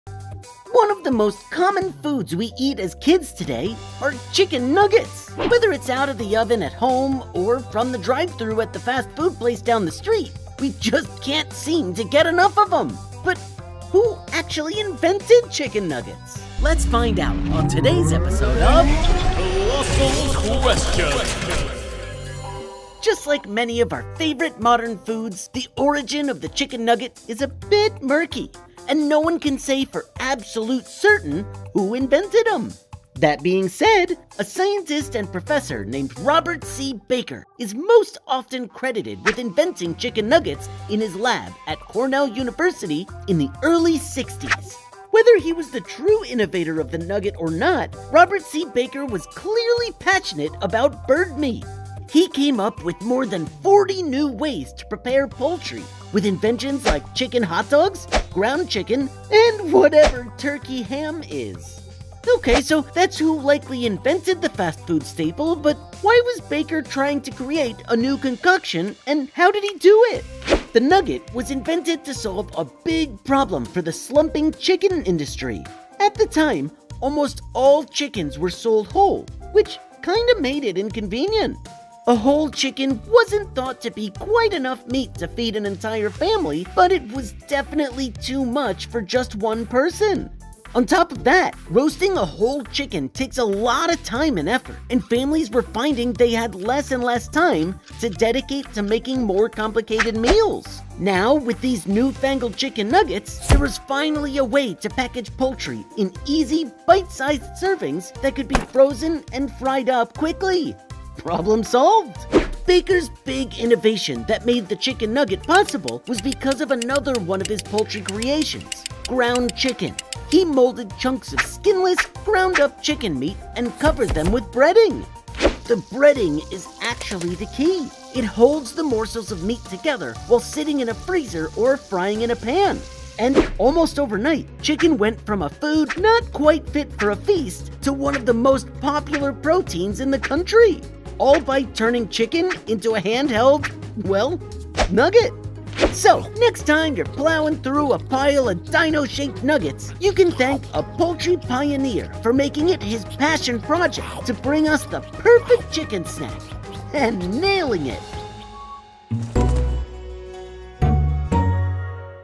Who Invented Chicken Nuggets? | COLOSSAL QUESTIONS
One of the most common foods we eat as kids today are chicken nuggets! Whether it’s out of the oven at home or from the drive through window at the fast food place down the street, we just can’t seem to get enough of ‘em! But... who actually invented chicken nuggets??

Written & Directed by: Matt Levy
Edited & Animated by: Jesse Henderson
Executive Producer: Judy Meyers